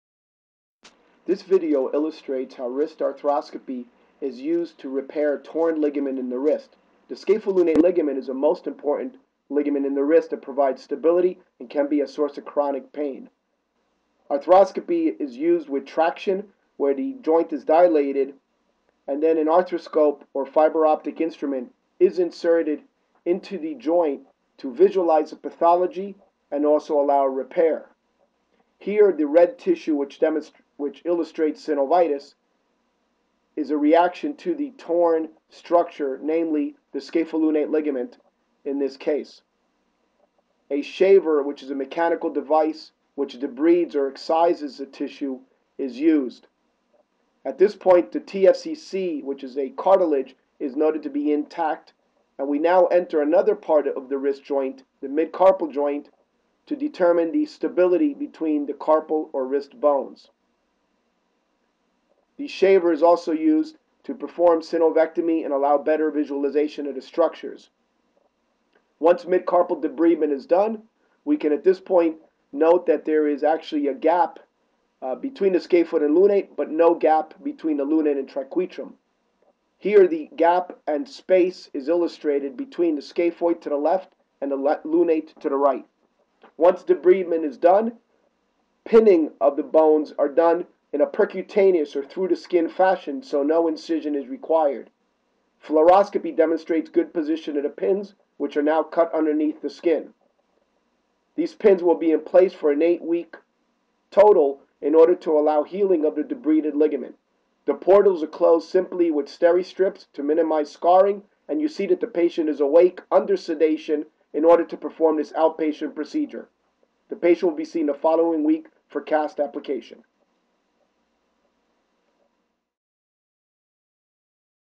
Scapholunate ligament Repair Wrist Surgery Center Dr. Alejandro Badia Wrist Injury Repair
This video illustrates an all arthroscopic technique for debridement and repair of a scapholunate ligament tear.